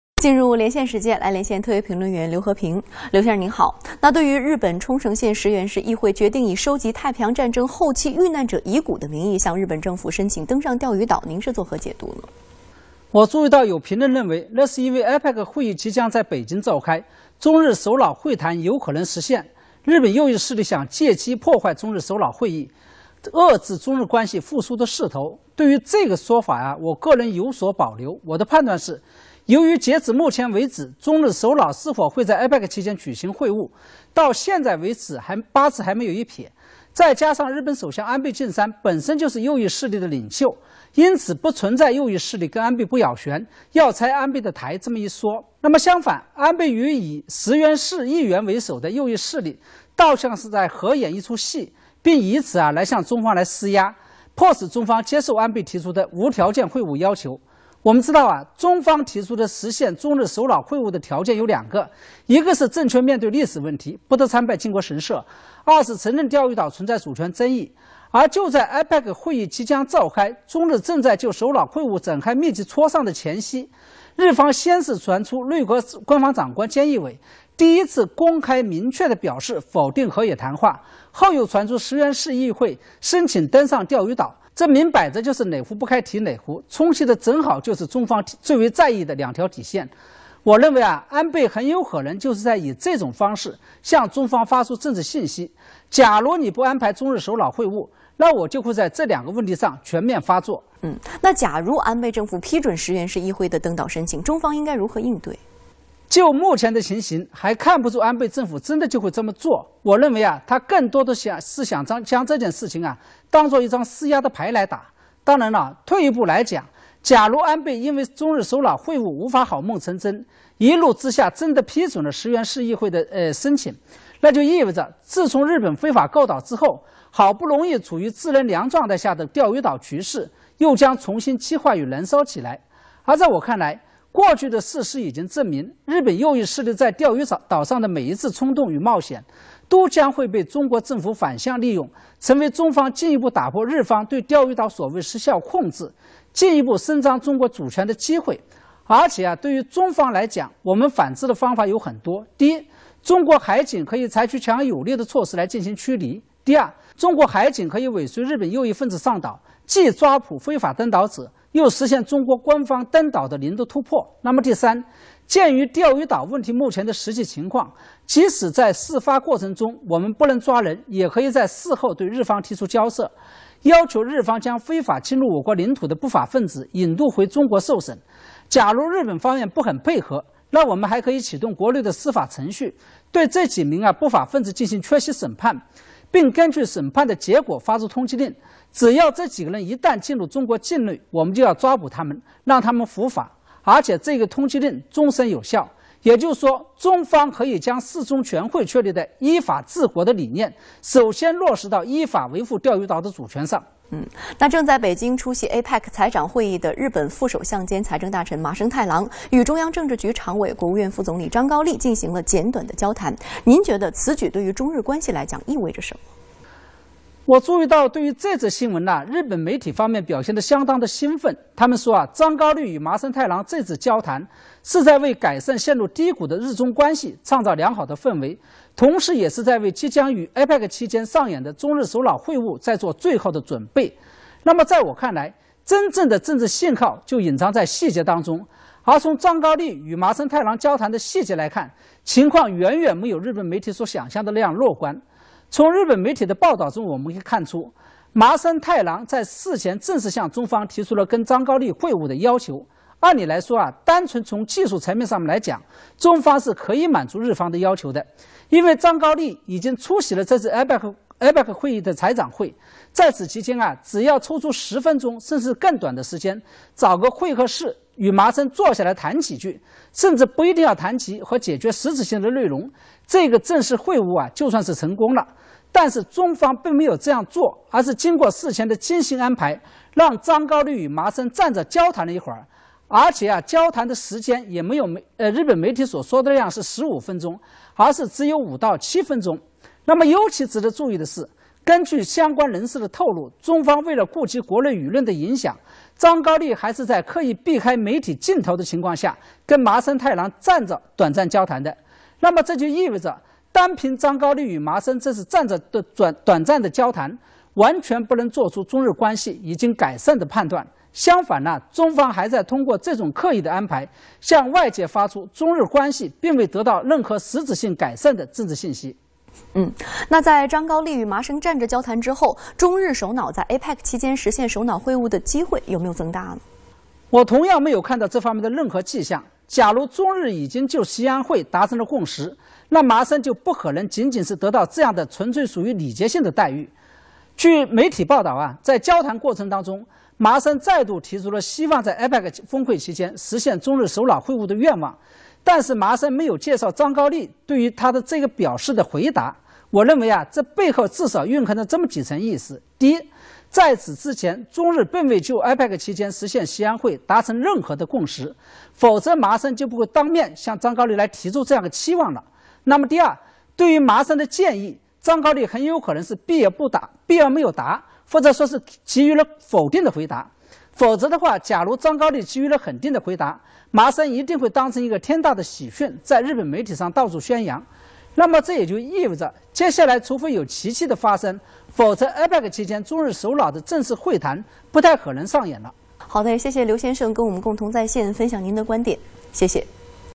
japanese councilmen board diaou island under cover of collecting remains 日多議員又登釣魚島
China News Network October 22 , according to Japanese news network reported on October 22 , Japan Okinawa Ishigaki City Council passed a resolution on the 21st , decided to collect the remains of the Pacific War in the name of the late asylum seekers to apply for registration of the Diaoyu Islands .

Resolution of the Parliament , said in July 1945 , in order to avoid war , and other residents of Ishigaki evacuate aboard vessels in the waters near Senkaku Islands were enemy fire, many survivors boarded the Senkaku Islands refuge , there have been 80 people died and other causes of death .

Senkaku Islands currently belongs to the jurisdiction of Ishigaki municipal government in the administrative jurisdiction of the resolution passed by the City Council believes that if it can not collect from some of the remains , then deal with the problem after the war never ended.

The Japanese government earlier regulations, without government approval may not be landing . 2005 , Ishigaki city government on behalf of the Government in the same application had landed on the island , has not been approved .

Although the Japanese government had rejected the Ishigaki municipal government put forward the so-called ” landing inspection” request, but Abe has repeatedly insisted that the government is still the Diaoyu Islands is ” Japanese territory .”

Earlier there were reports that there is an elevation of 526 meters in Mau Dengyue on Ishigaki Ishigaki City . Standing on a hill overlooking the sea to the west , 170 km away is China ‘s Diaoyu Islands . Geographically, compared to the distance and the Japanese mainland , Ishigaki and China Taiwan closer, just over 200 kilometers away in Taiwan .
japanese councilmen board diaou island under cover of collecting remains 日多議員又登釣魚島 ,for more information about china world news